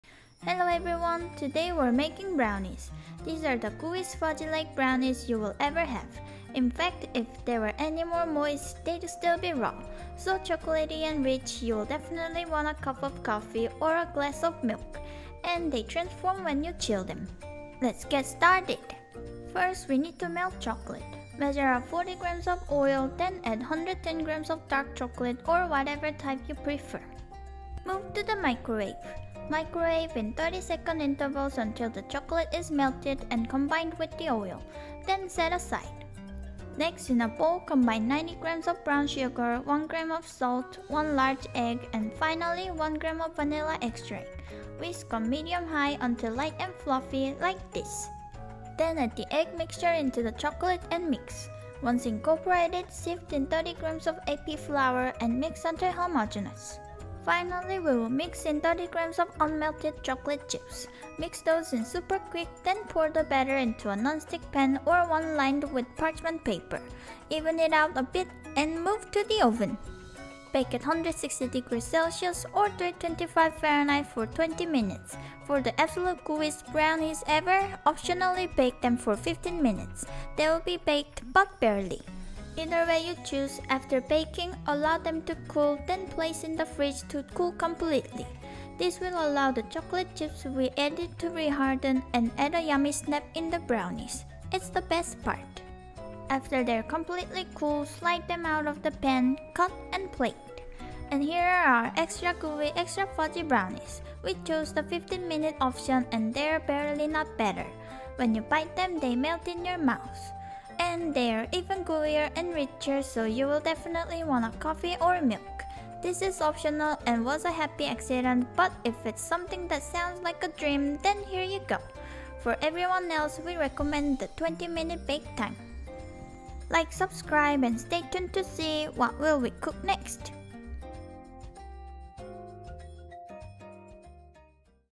Extra Gooey Fudgy Brownies! It Just Melts In Your Mouth!
These brownies are the most rich and gooey I've ever had!

So rich and gooey, they just melt in your mouth.

If you want extra gooeyness bake them for only 15 minutes. It will be baked but barely!

* Ingredients
- Dark Chocolate 110g
- Oil 40g
- Brown Sugar 90g
- Salt 1g
- Egg large 1ea
- Vanilla Extract 1g
- AP Flour 30g
- Chocolate Chips 30g

* Method
1. Melt dark chocolate and oil in the microwave.
2. Whisk sugar, salt, egg, and vanilla extract until light and fluffy.
3. Mix the egg mixture into the chocolate.
4. Sift in flour and mix until homogenous.
5. Add chocolate chips and mix in quick.
6. Pour the batter into a nonstick pan or one lined with parchment paper.
7. Bake at 160c or 325f for 20 minutes.
8. Cool then place in the fridge to cool completely.
9. Cut it into the size and shape you want and enjoy!